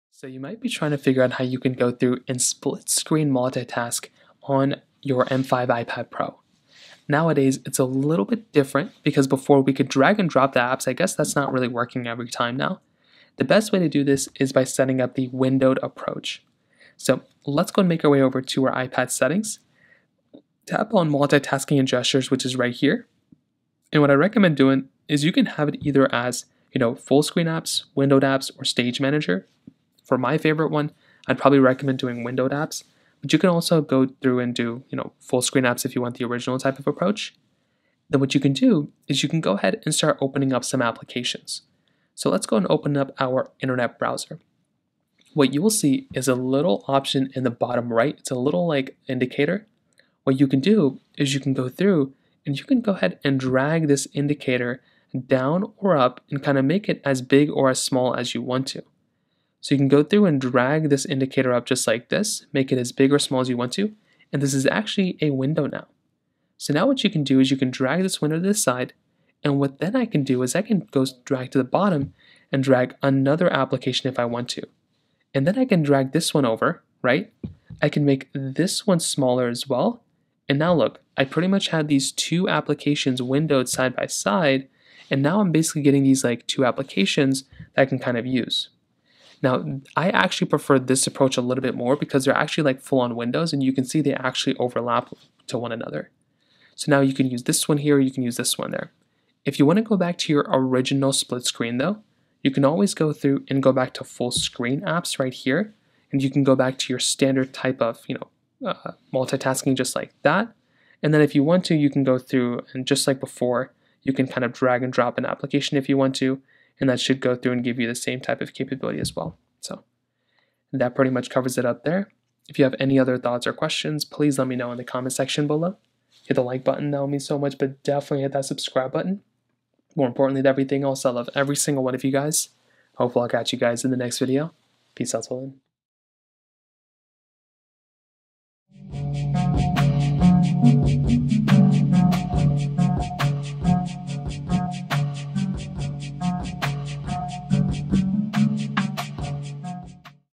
How To Split Screen Multitask On M5 iPad Pro!
Here is exactly How To Split Screen Multitask On M5 iPad Pro!